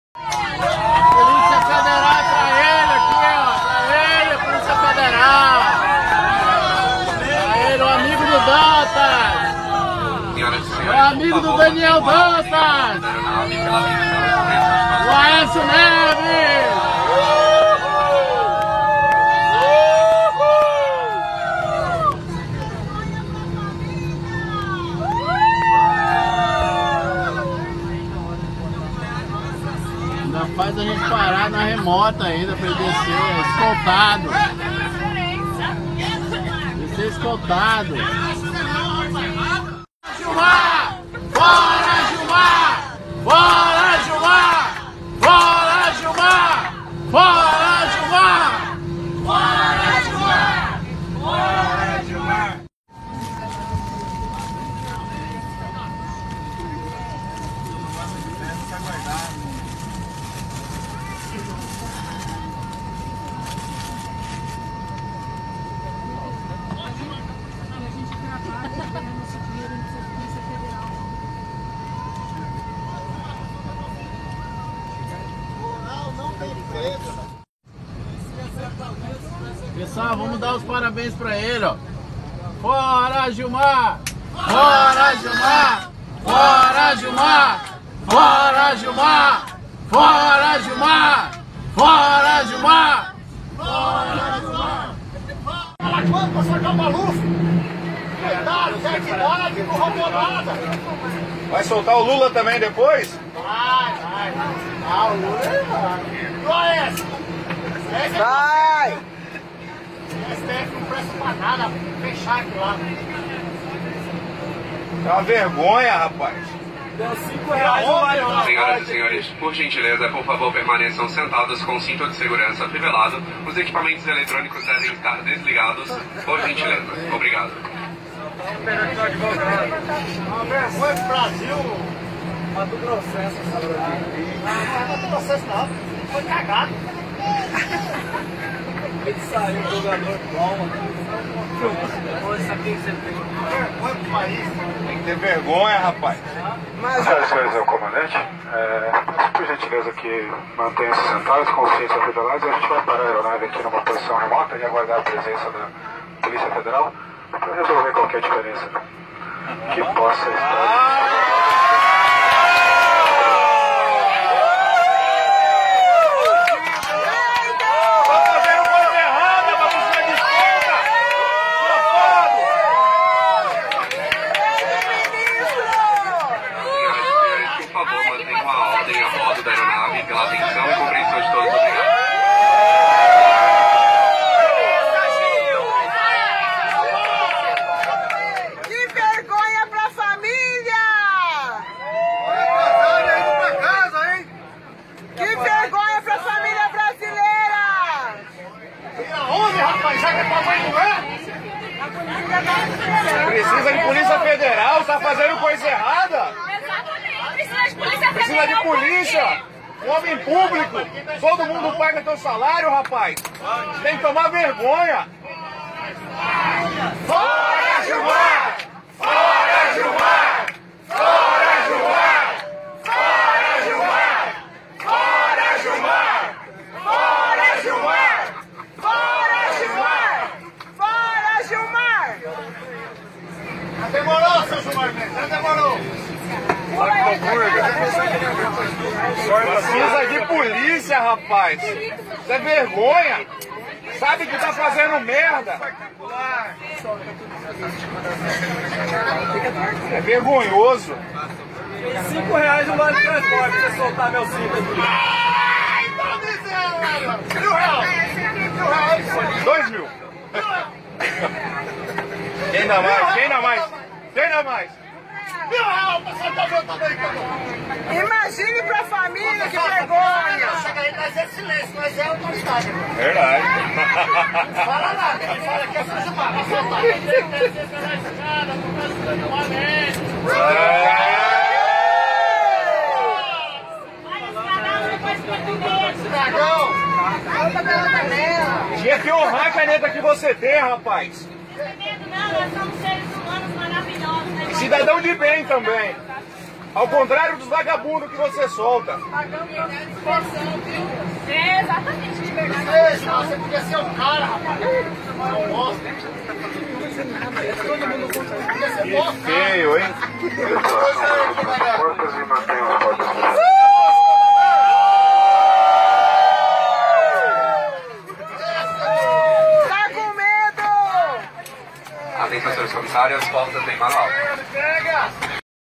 Gilmar Mendes hostilizado em avião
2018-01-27 - Gilmar Ferreira Mendes (nascido em 30 de Dezembro de 1955), Ministro do Supremo Tribunal Federal (STF) e Presidente do Tribunal Superior Eleitoral (TSE), foi fortemente hostilizado em vôo entre Brasília e Cuiabá.

Pouco tempo após retorno ao Brasil de viagem para Lisboa, Portugal, na qual também sofreu agressões verbais em manifestações contrárias a seus comportamentos recentes tanto no TRE, quanto no STF.

Gilmar recusa implementar o voto impresso na próxima eleição, apesar de haver recursos disponíveis, e tem soltado repetidas vezes aliados corruptos como Jacob Barata Filho.